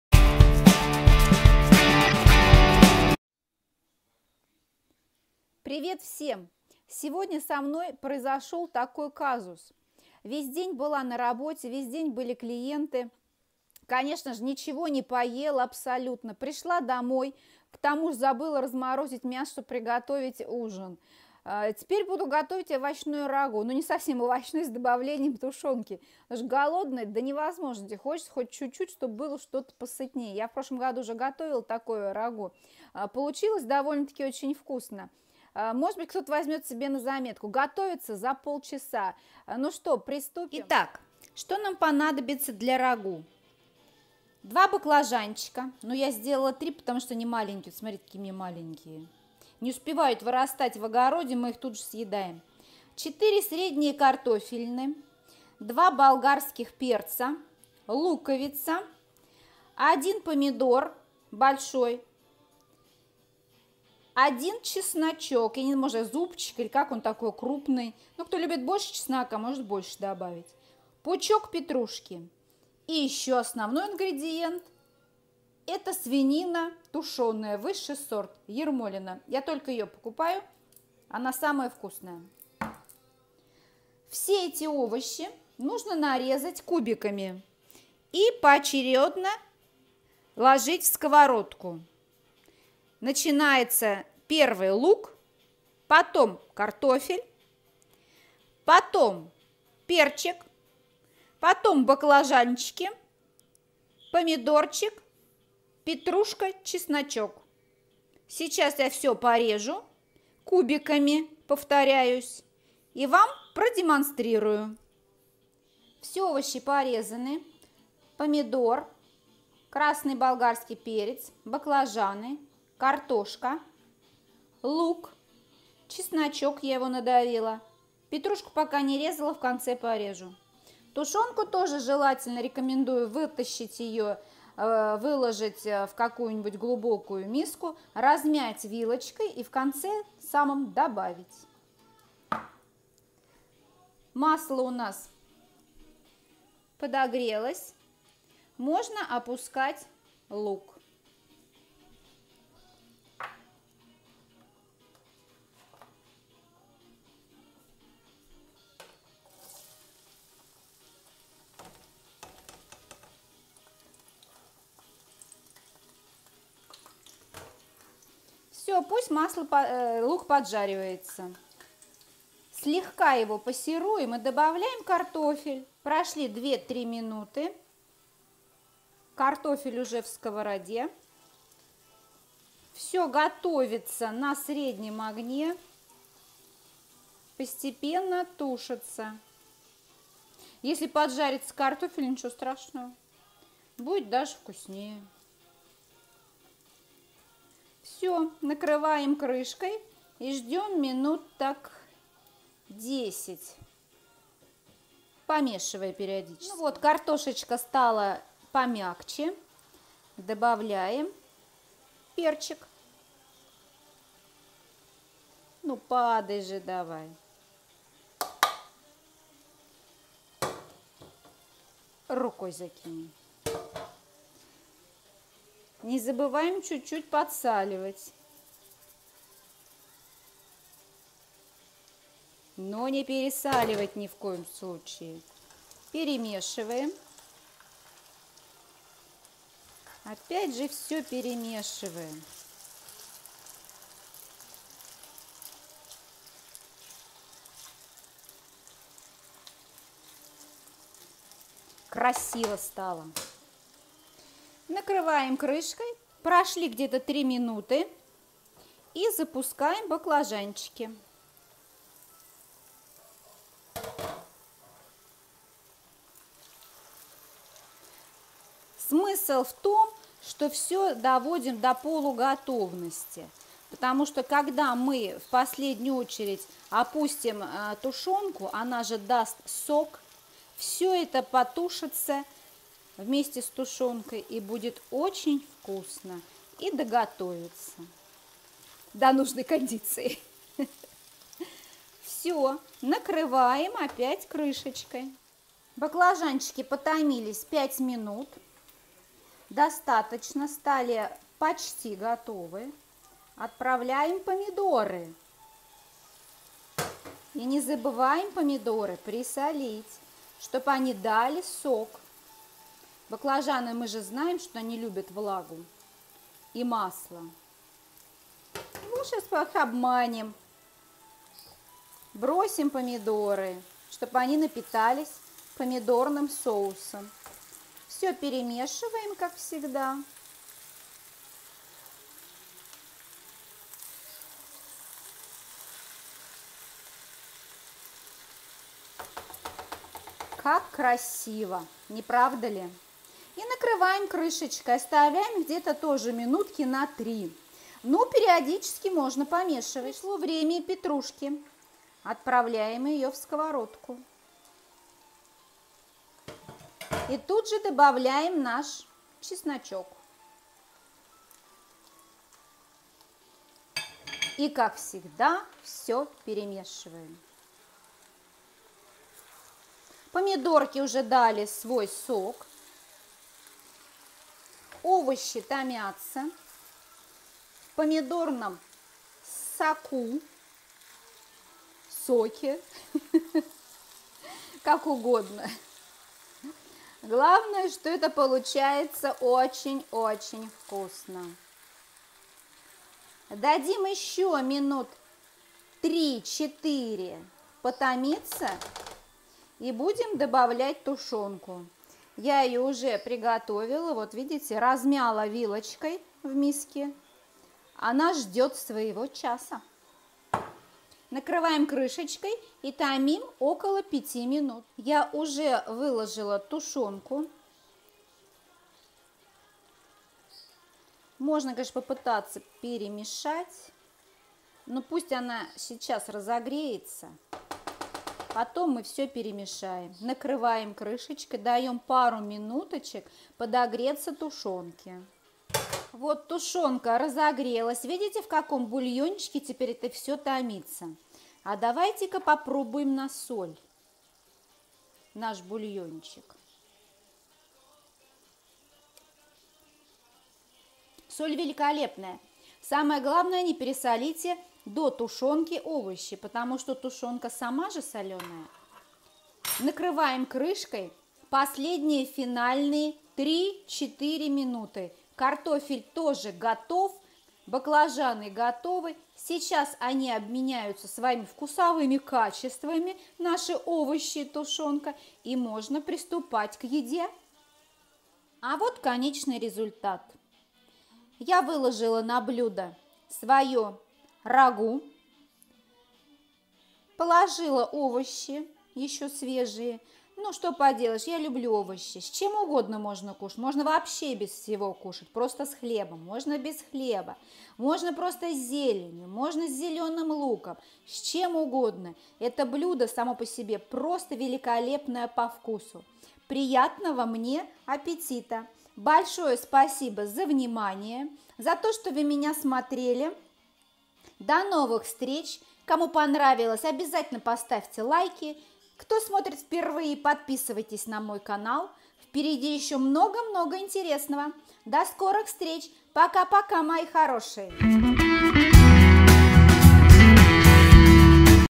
Овощное РАГУ на скорую руку!!!
Рецепт
2 средних баклажана
1 луковица
4 средних картофилины
2 зубчика чеснока
1 большой помидор
пучок петрушки
Можно добавить морковь